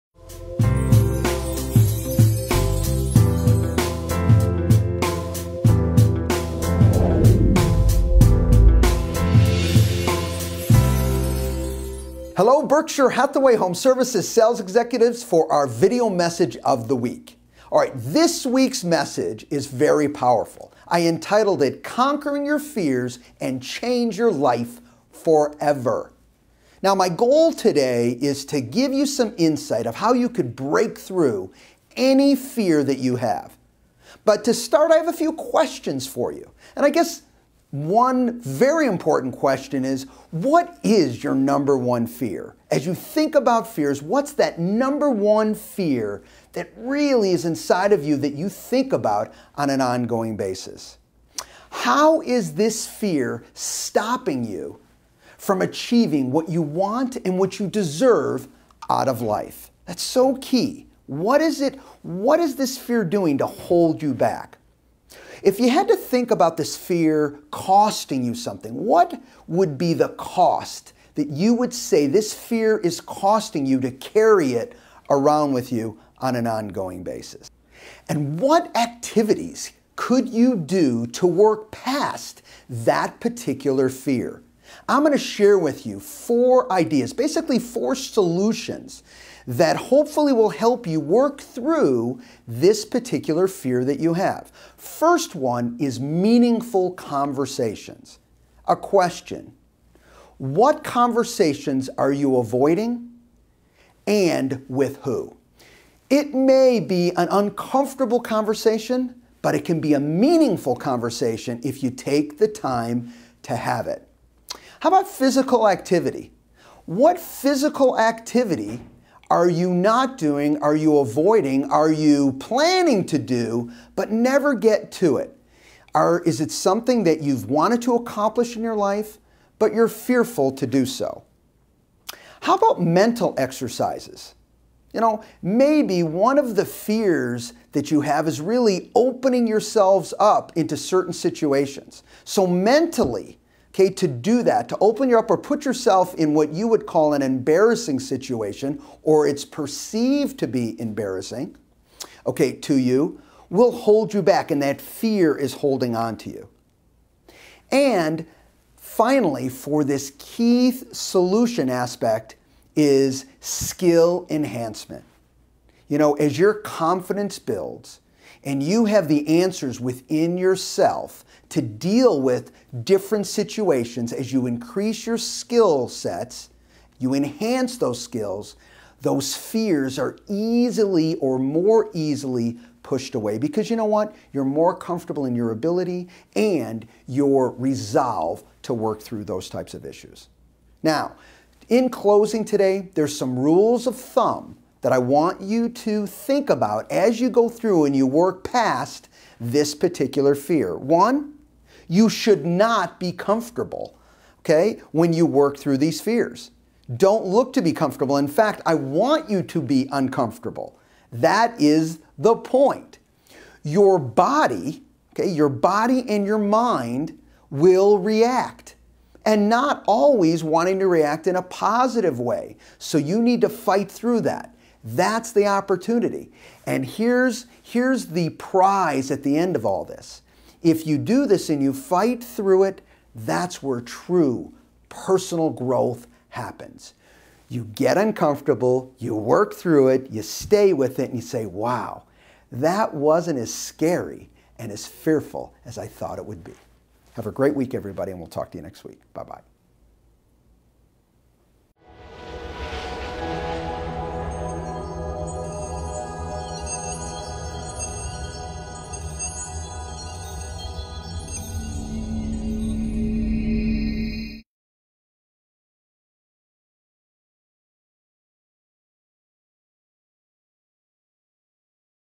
Real Estate Training - Conquer Your Fears And Change Your Life Forever
Conquer your FEARS and change your Life Forever!

• What is your #1 Fear?

•  How is this Fear stopping you from getting all you WANT and DESERVE out of life?

•What is the Cost of Carrying this Fear around with you?

•What activities could you do to help yourself concur this fear?
-Meaningful Conversations
-Physical Activity
-Mental Exercises
-Skill Enhancement

•Rules of Thumb

-You should NOT be comfortable doing these actions

-Your body & mind will react

-This is when true personal growth occurs